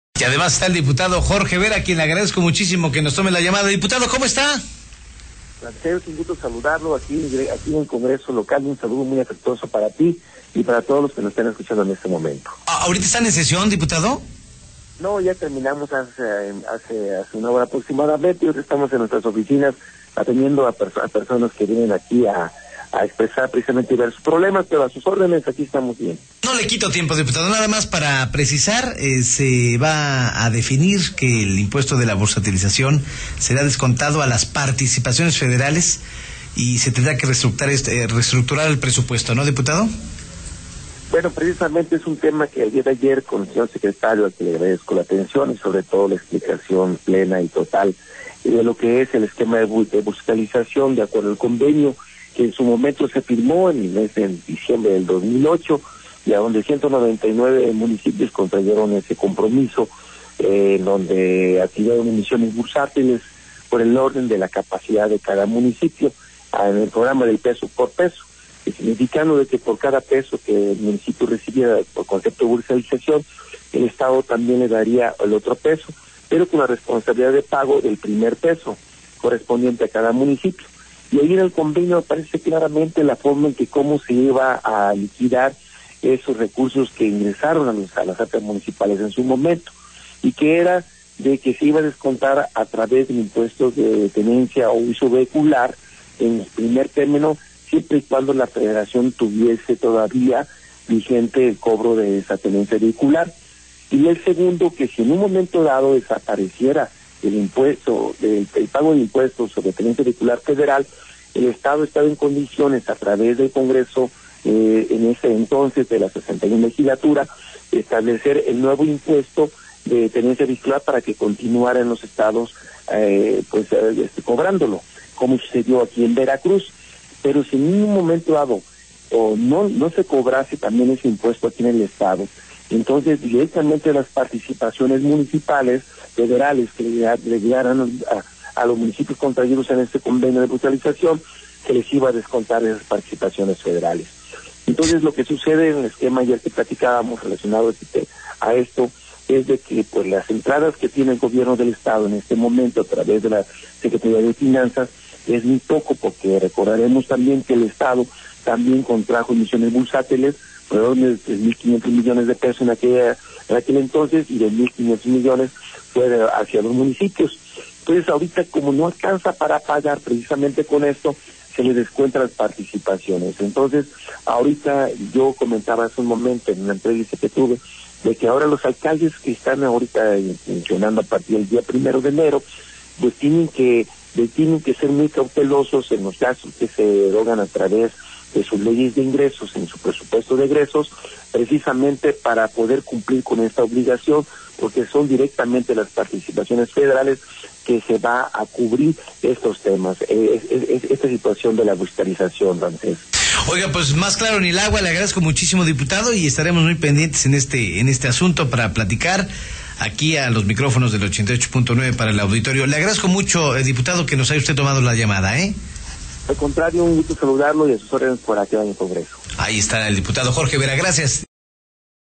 Uni Noticias 88.9 fm: Entrevista a Jorge Vera Hernández
Entrevista realizada por el periodista Ramsés Yunes Zorrilla a Jorge Vera Hernández, diputado local del PAN, con motivo de la implementación de reglas de operación de bursatiliazión para los ayuntamientos que hayan entrado al programa. (16/01/2014)